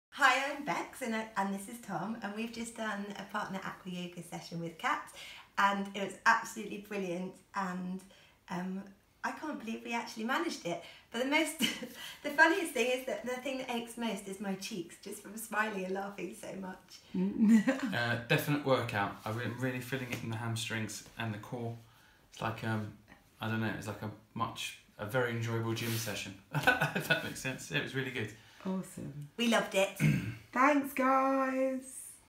What to expect from Partner and Acroyoga 121’s
A little testimonial from lovely couple who have been doing private Acroyoga and partner yoga sessions with me in my home studio. Next week is their Thai Massage session where they’ll go home having learned to give and receive a sequence to eachother. I’m doing a special seasonal offer of 3 for 2 on bookings right now. Offer is limited so fly this way if you are looking to share a Christmas pressie together with your partner 😉